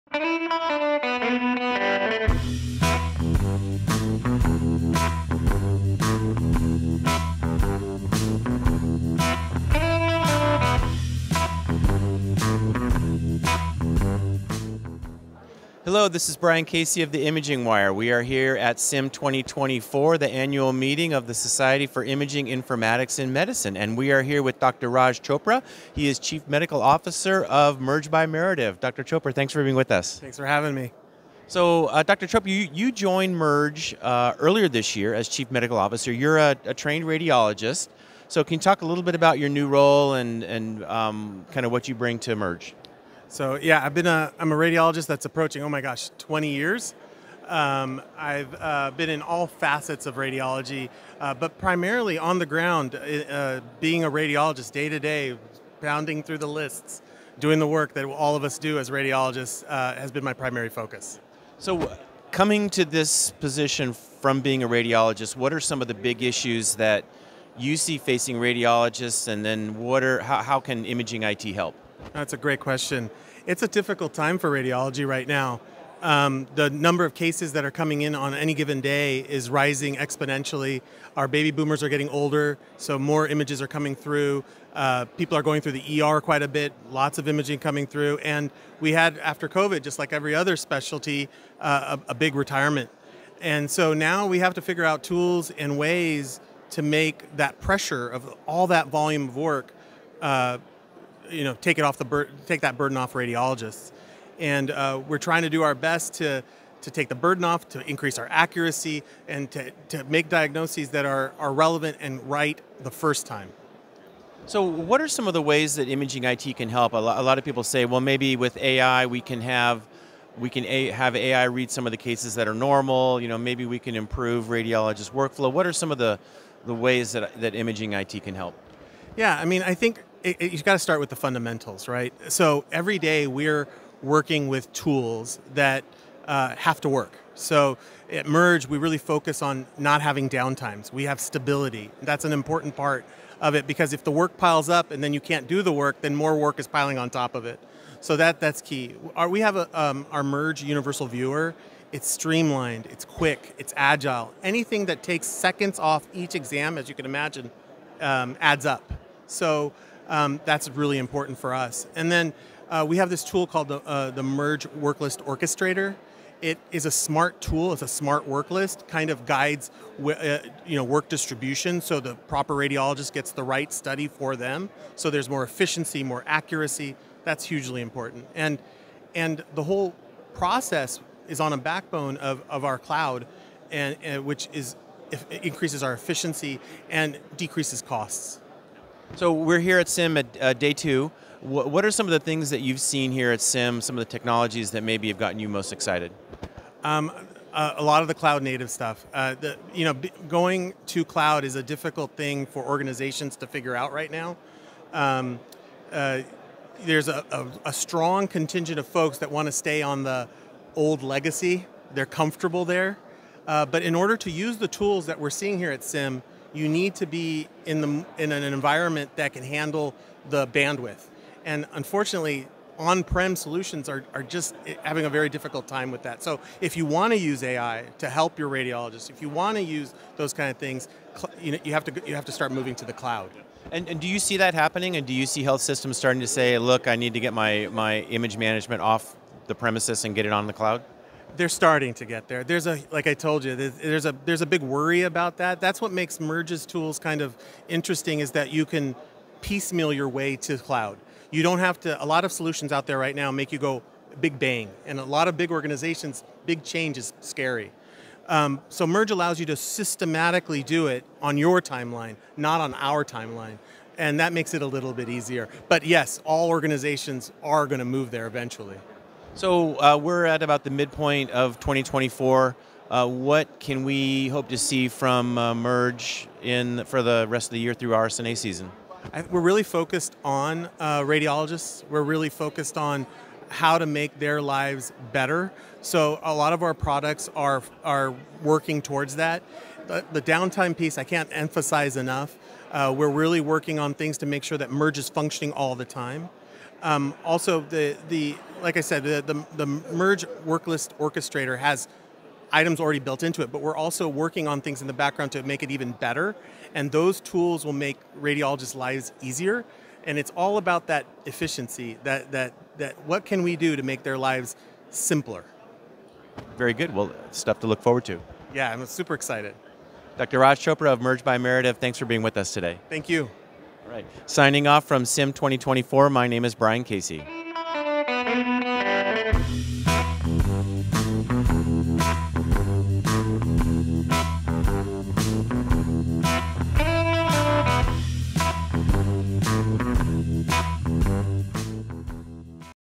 Interview at SIIM 2024 -- Merge by Merative
We talked to Raj Chopra, MD, of Merge by Merative at SIIM24 about his new role as Chief Medical Officer at Merge, and what he sees as major trends at the show.